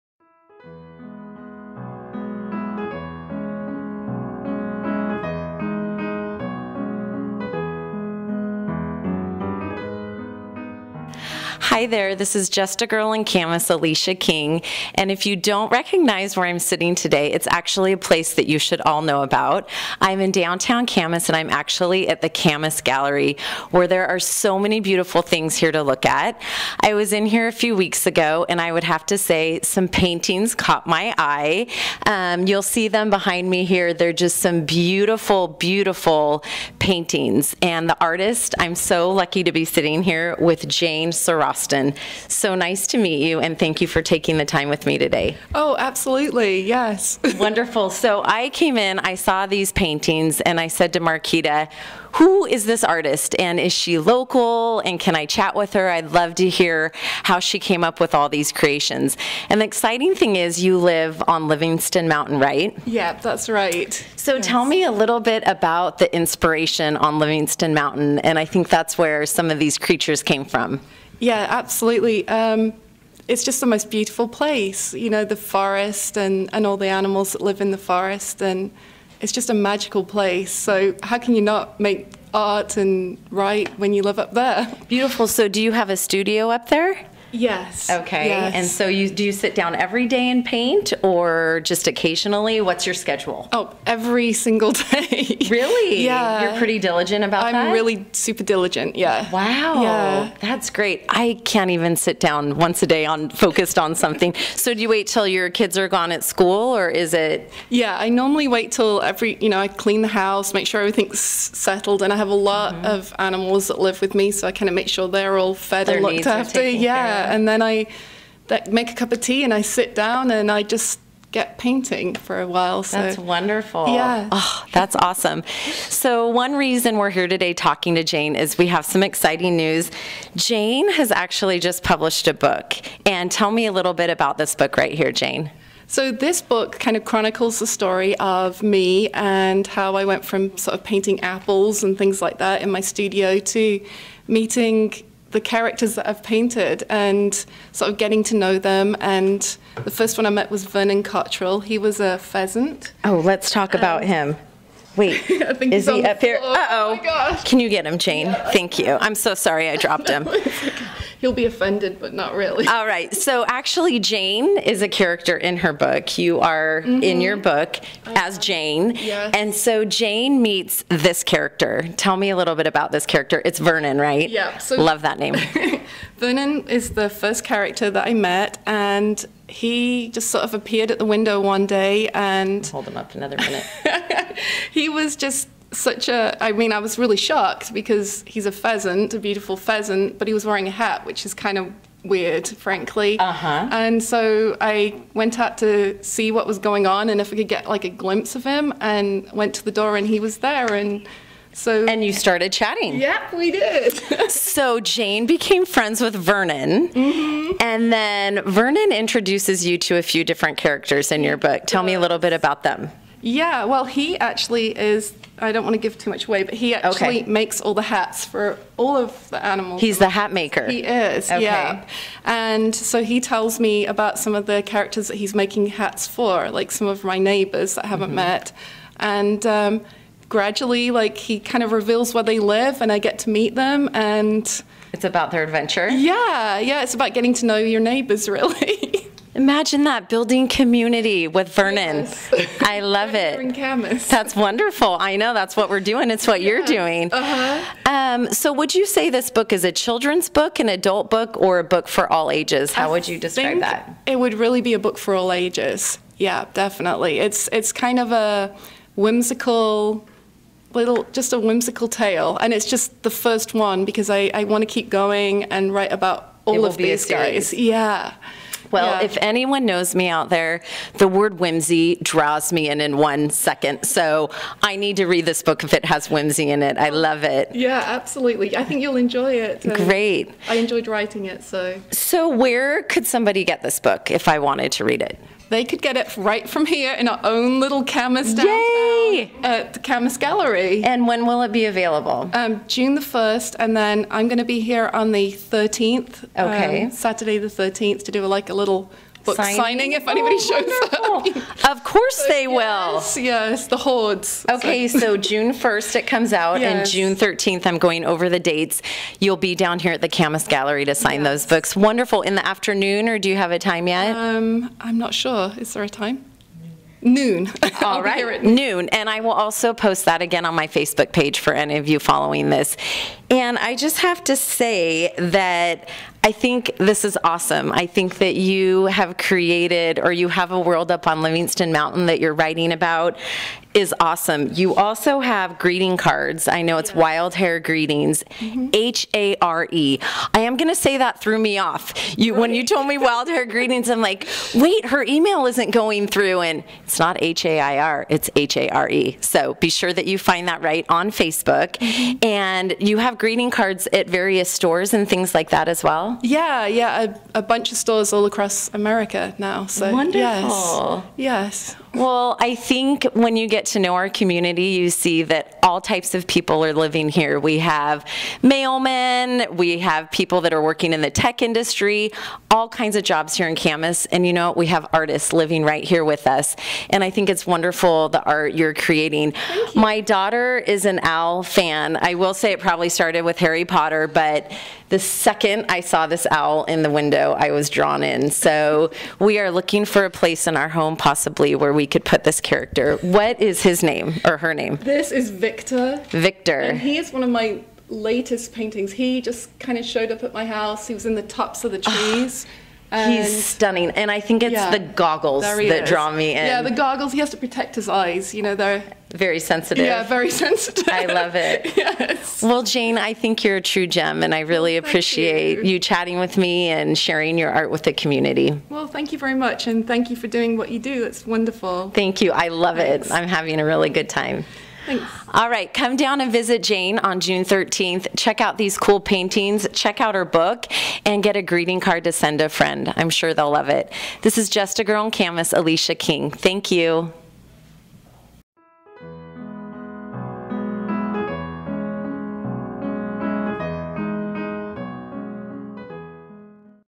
Local Camas Artist Jayne Siroshton
Check out this fabulous local artist Jayne Siroshton.  She has beautiful art featured in the Camas Gallery and has just come out with a book!  Hear about her book signing and when and where you can get your copy signed!  This city is full of so much talent.